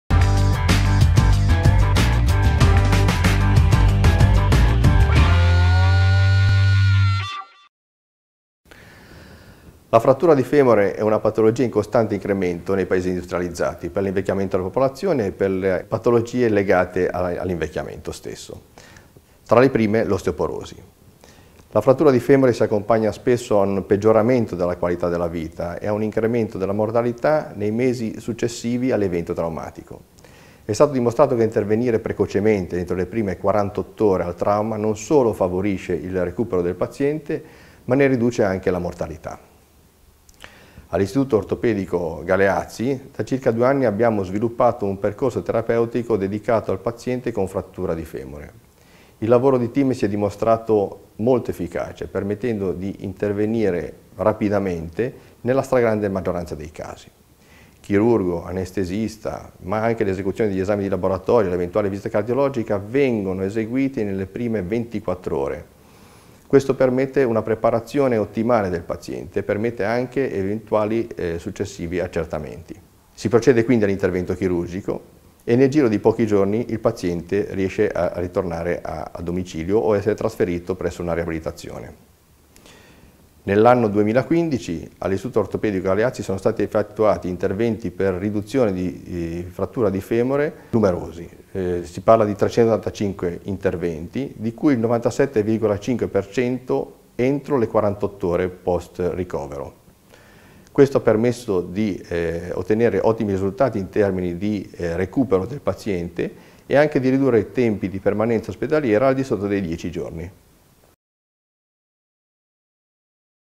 La risposta dell'Esperto - Terapia intensiva post operatoria: trattamento delle fratture di femore
Il dottor Paolo Perazzo, responsabile dell'Unità Operativa di Terapia Intensiva Post Operatoria Anestesia e Rianimazione all'IRCCS Istituto Ortopedico Galeazzi​, spiega quanto sia importante intervenire tempestivamente in caso di frattura al femore